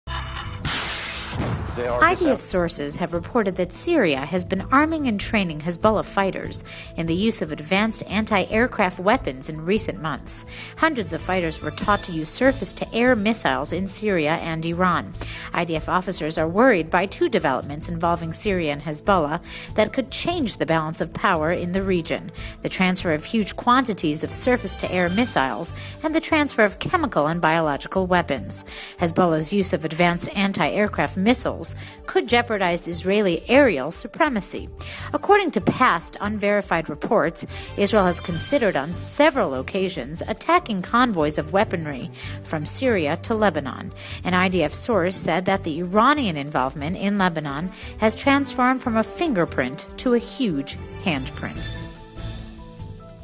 IDF worried about Syria-Hezbollah relationship
IDF sources have reported that Syria has been arming and training
Hezbollah fighters in the use of advanced antiaircraft weapons in
recent months.  Hundreds of fighters were taught to use surface-to-air
missiles in Syria and Iran. IDF officers are worried by two
developments involving Syria and Hezbollah that could change the
balance of power in the region: The transfer of huge quantities of
surface-to-air missiles; and the transfer of chemical and biological
weapons. Hezbollah's use of advanced antiaircraft missiles could
jeopardize Israeli aerial supremacy. According to past, unverified,
reports, Israel has considered on several occasions attacking convoys
of weaponry from Syria to Lebanon. An IDF source said that the Iranian
involvement in Lebanon has transformed from a "fingerprint" to "a huge
handprint."